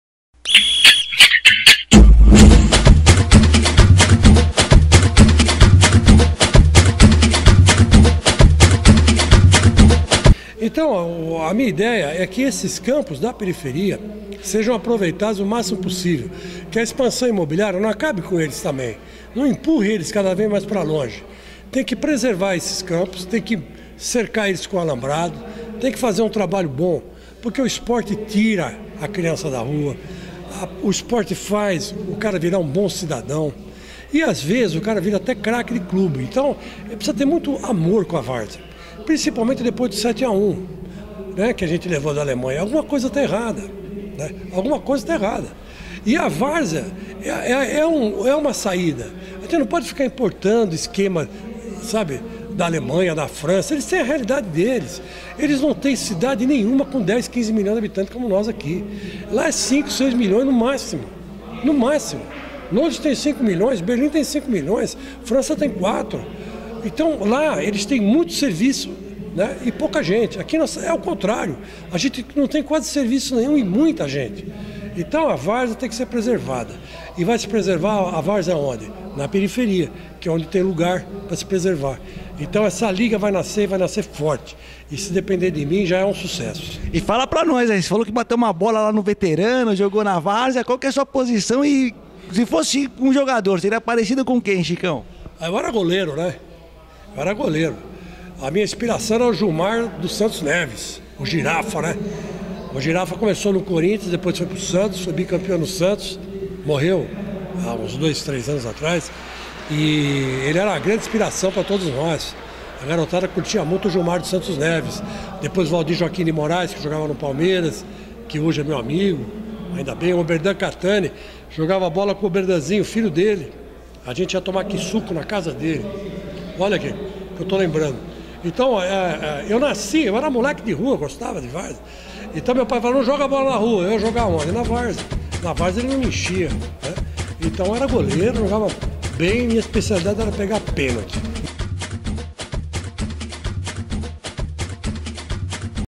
Fragmentos da Várzea - Fica a dica (Futebol de Várzea)
Depoimento de Chico Lang no congresso técnico da 1ª Copa Libertadores da Várzea (LOCFA), 31/07/2014